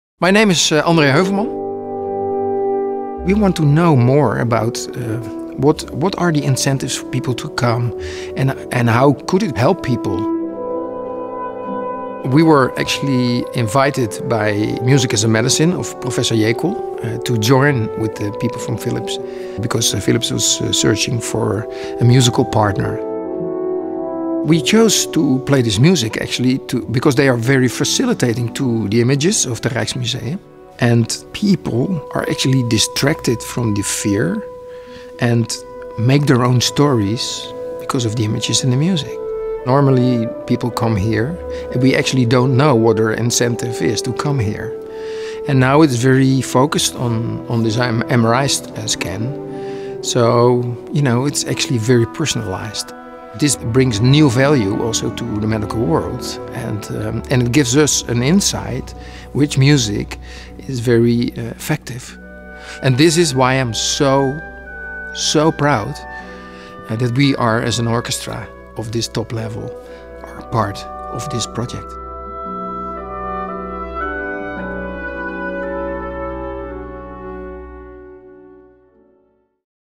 Dutch Masters | Rotterdam Philharmonic Orchestra
Zooming in on music. A collaboration between Philips, Rijksmuseum, Rotterdam Philharmonic Orchestra, AMC and Erasmus Medical Center has led to an exciting new Ambient Experience theme. Together we show how art and music can improve the patient experience.